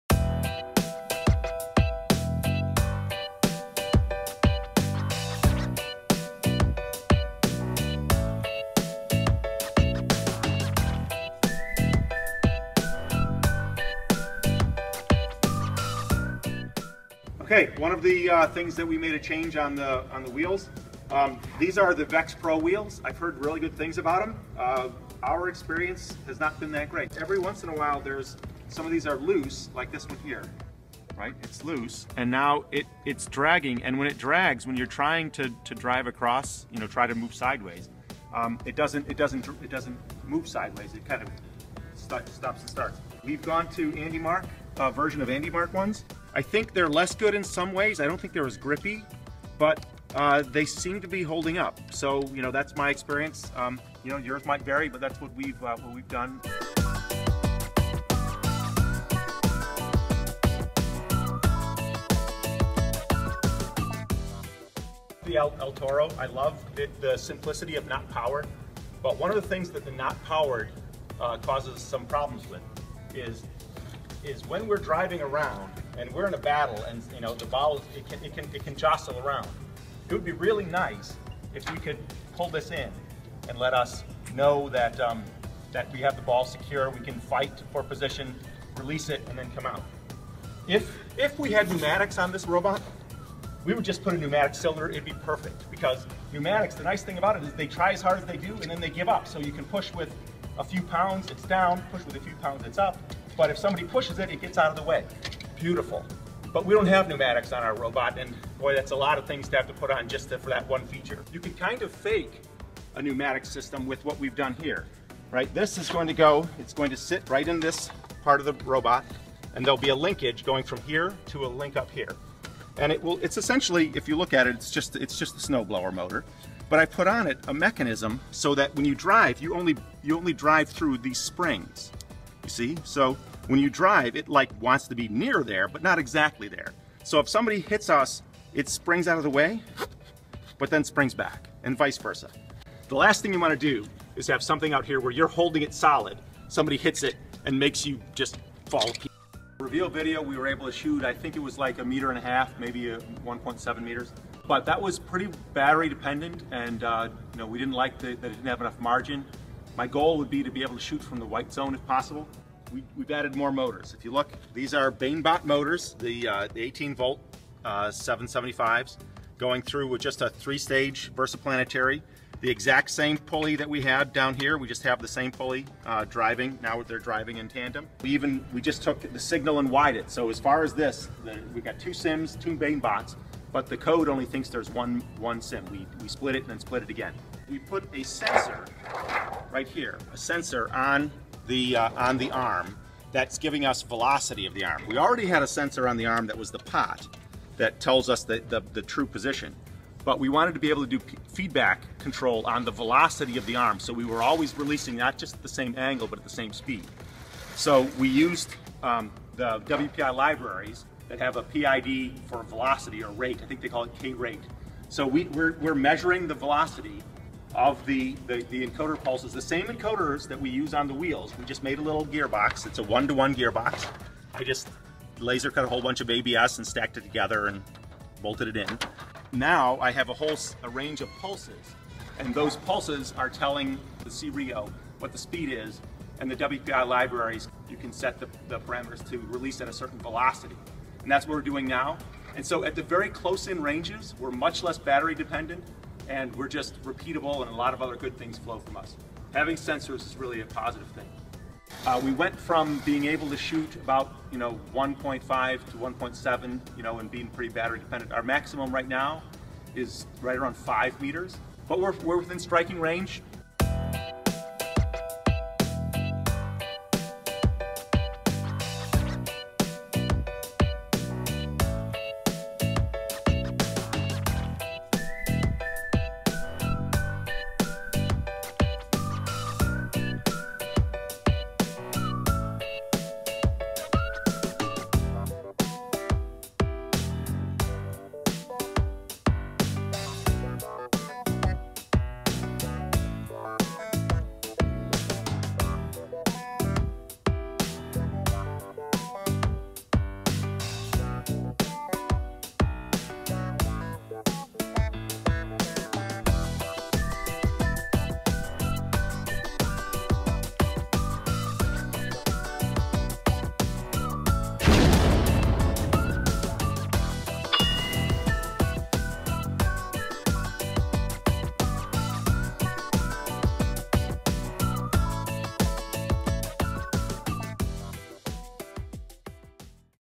Ri3D: Boom Done - El Toro Improvements
Team Boom Done has made a few improvements to El Toro in the past few weeks and really extend the shooting range of the robot. Dr. Joe just couldn't wait to show them to you. We hope your build season is going well!